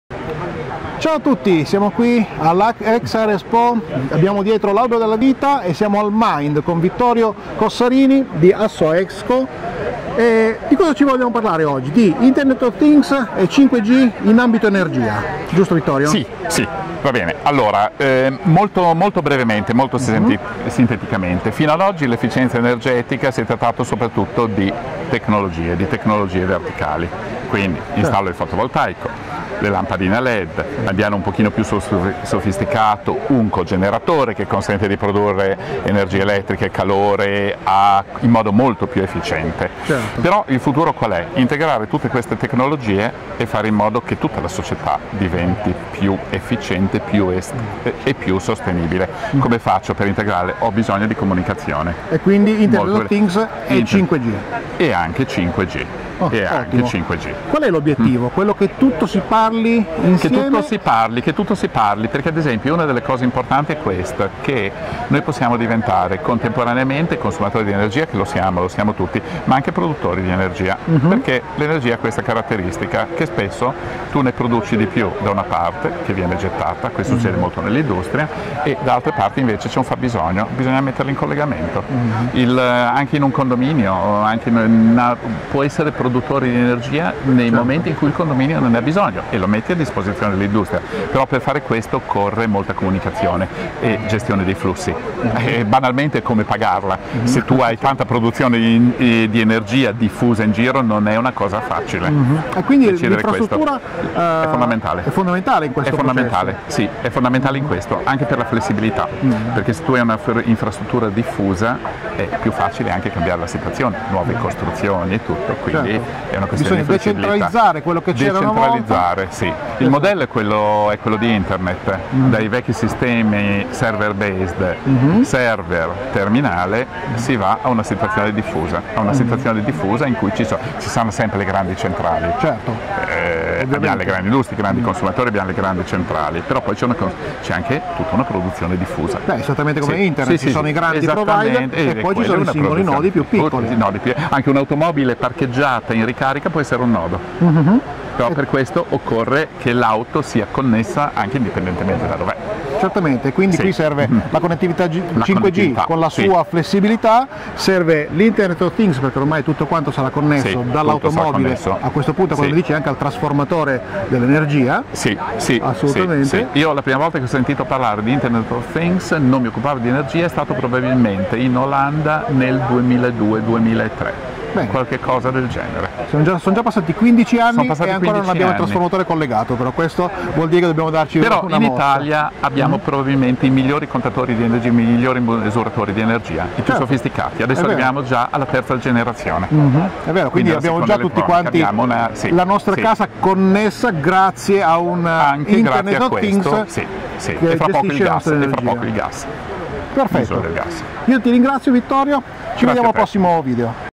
Aspettando IOTHINGS. Intervista a Vittorio Cossarini
Intervista a Vittorio Cossarini, Comitato Tecnico Comunicazione, ASSOESCo durante la presentazione di IOTHINGS Milan 2019 presso MIND